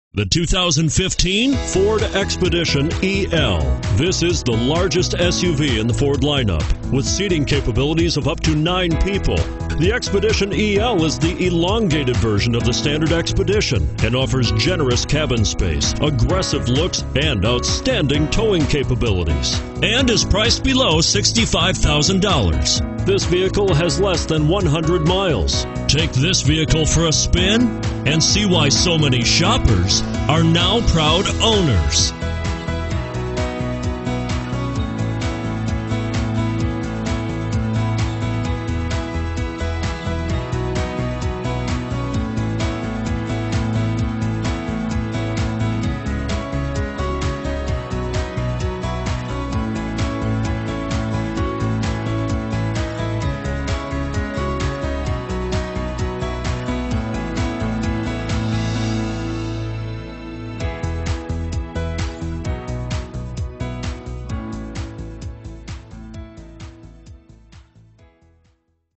2015 Ford Expedition EL Clear Lake Mason City FF051
2015 Ford Expedition EL Limited - $63360

Pick up this BRONZE_FIRE_METALL 2015 Ford Expedition EL, available today at Community Mason City Ford Lincoln. Featuring Automatic transmission, it has 5 miles. This could be the one you've been searching for.